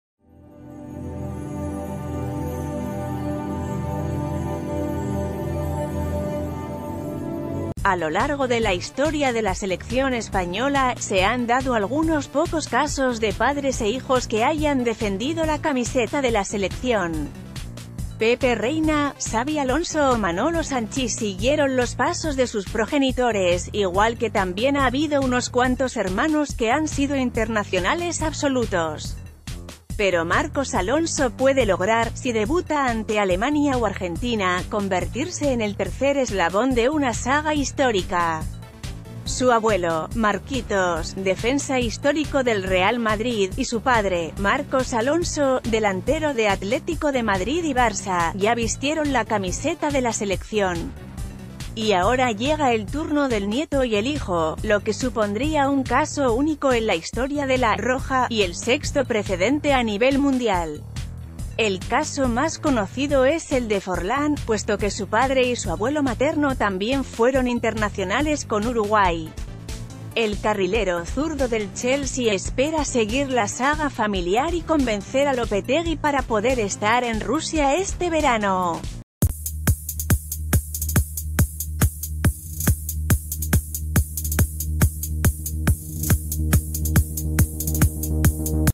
Noticias de última hora | Marcos Alonso, el tercer eslabón de una saga histórica
A lo largo de la historia de la selección española, se han dado algunos -pocos- casos de padres e hijos que hayan defendido la camiseta de la selección. Pepe Reina, Xabi Alonso o Manolo Sanchís siguieron los pasos de sus progenitores, igual que también ha habido unos cuantos hermanos que han sido internacionales absolutos. Pero Marcos Alonso puede lograr, si debuta ante Alemania o Argentina, convertirse en el tercer eslabón de una saga histórica. Su abuelo, Marquitos, defensa histórico del Real ...